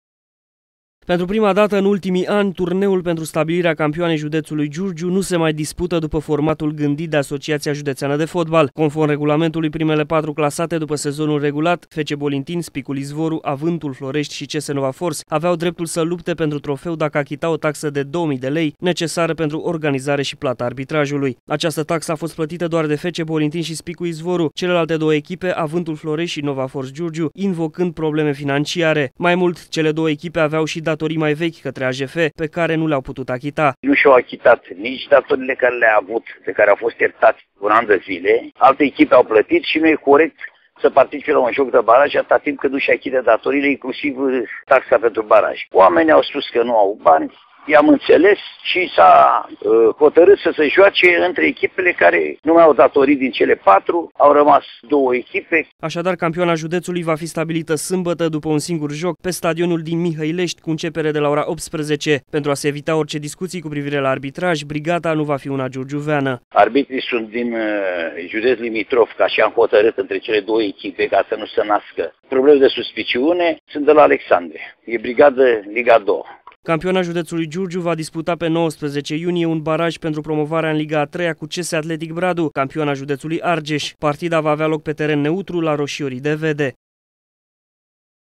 Turneul s-a transformat în finală
Surpriză de proporţii în fotbalul giurgiuvean înainte de stabilirea campioanei judeţului. Gândit iniţial ca un turneu la care să participe primele patru clasate la finele sezonului regulat, sistemul a fost transformat peste noapte într-o finală între FC Bolintin şi Spicul Izvoru. Decizia a fost luată de reprezentanţii AJF Giurgiu după ce avântul floreşti şi CS Nova Force nu şi-au plătit restanţele către asociaţie şi nici nu au achitat taxa de participare la baraj.